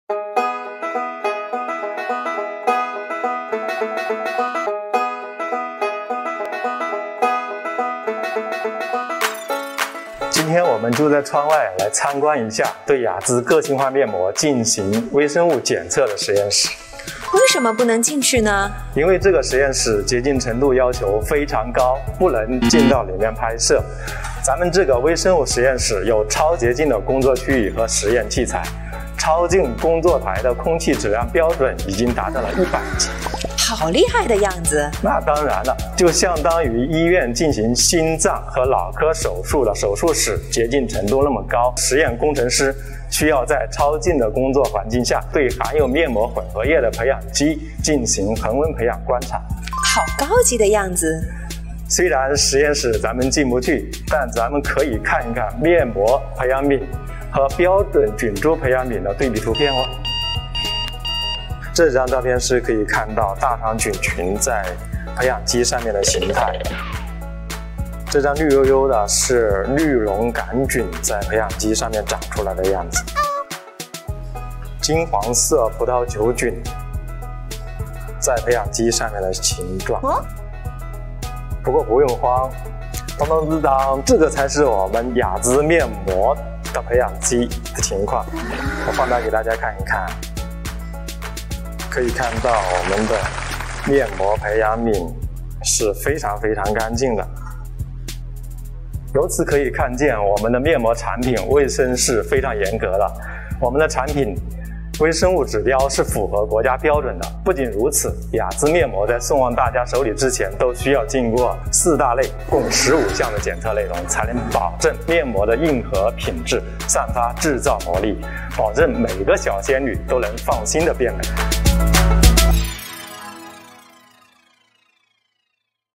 厉害了安利 ～ 雅姿个性化系列面膜微生物检测
在安利微生物实验室 ，我们会在空气质量达100级的洁净台中，对雅姿个性化系列面膜进行全面微生物检测，严格杜绝微生物超标对皮肤造成的感染危害。